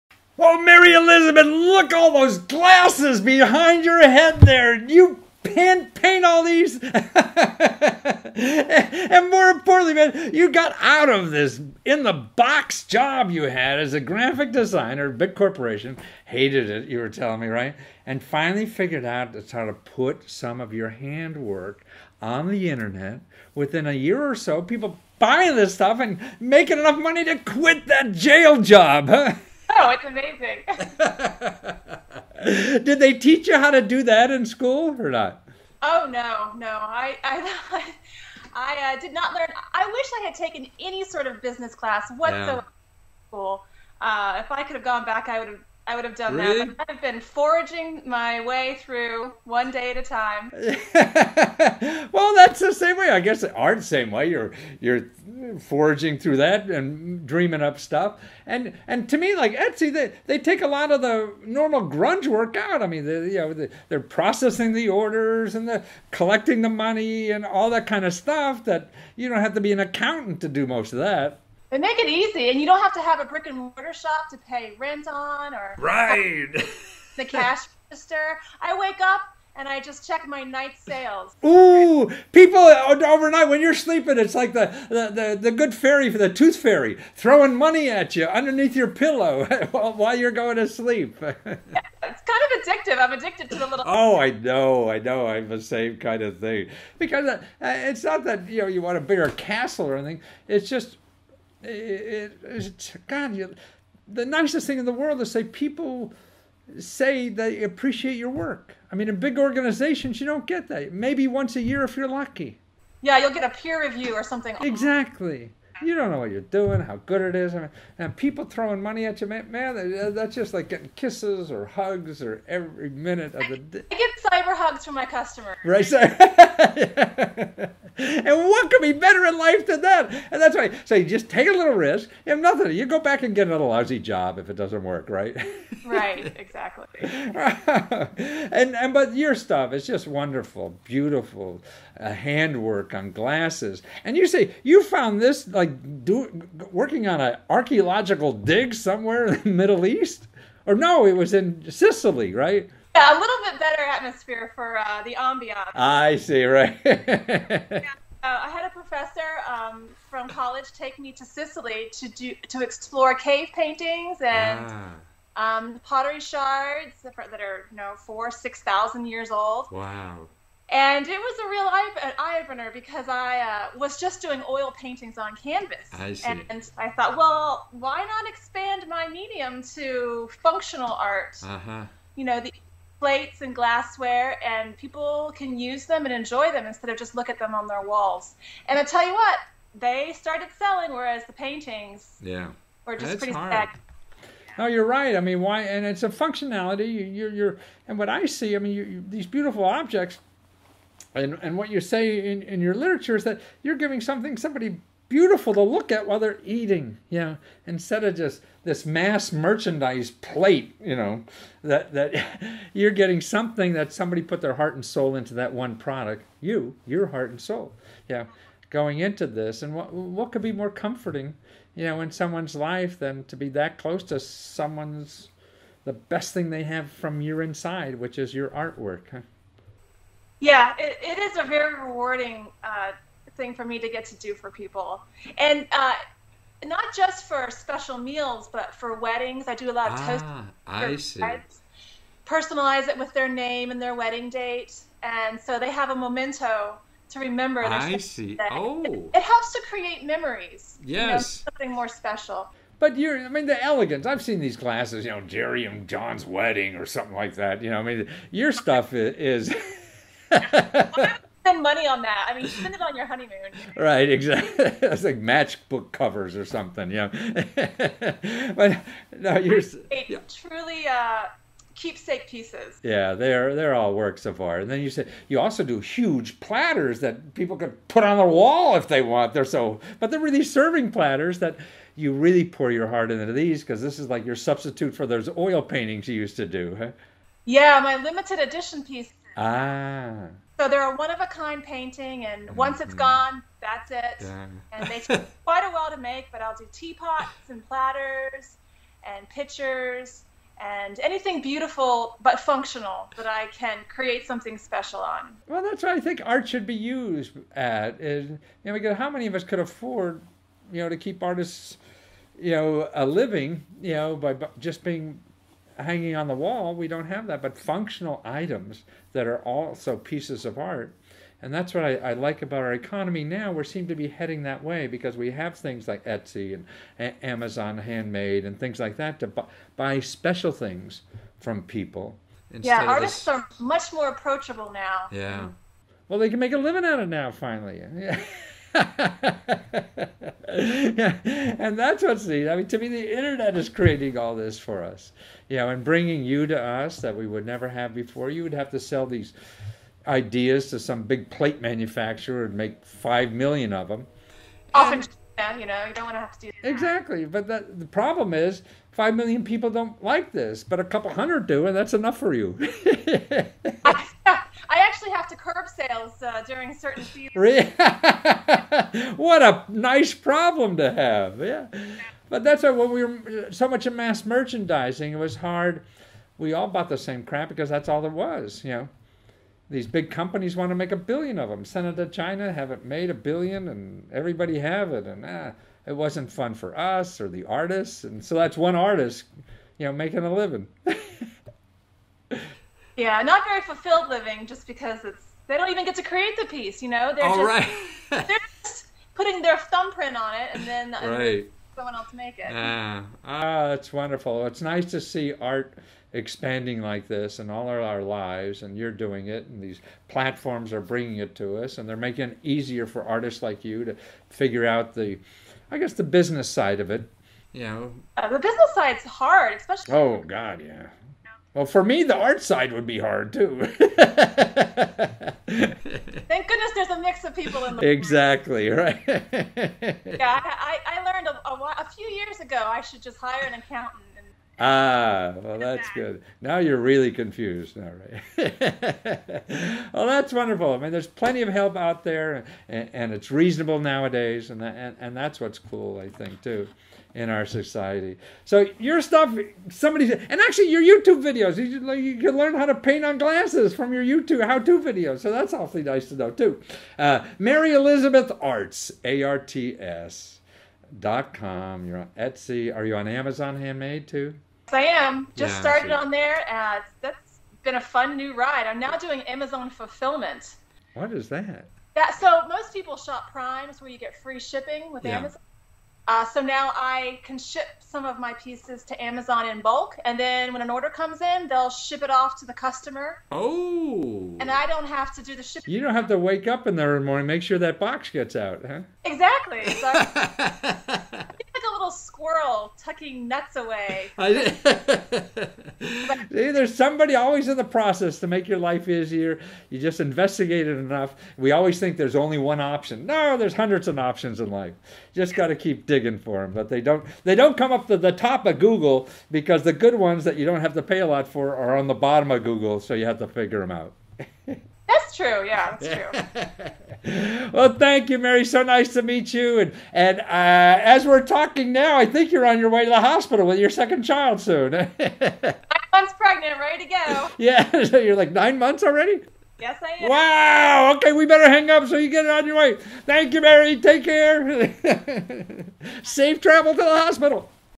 It Took One Year With Part Time INternet Store Until She Could Quit Her Boring Job
Matthew Lesko of interviews Mary Elizabeth of about how it took her a year with her part time internet store until she quit her boring job

Support Mary Elizabeth